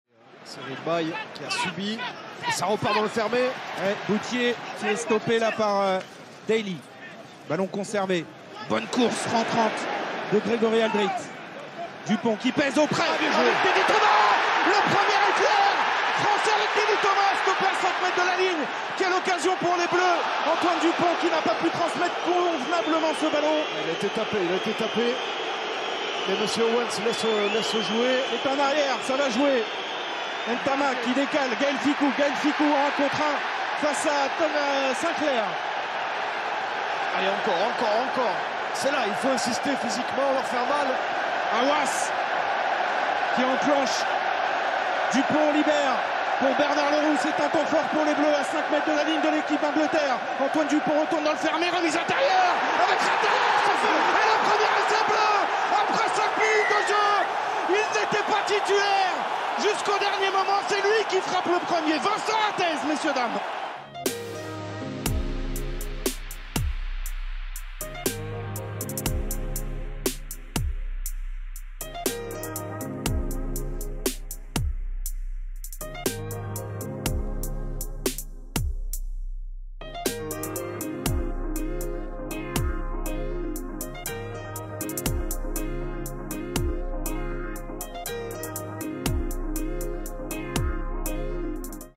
France - Angleterre (2020) : Vincent Rattez
Revivez l'essai marqué par Vincent Rattez lors du dernier tournoi des 6 Nations, à l'occasion du match d'ouverture contre l'Angleterre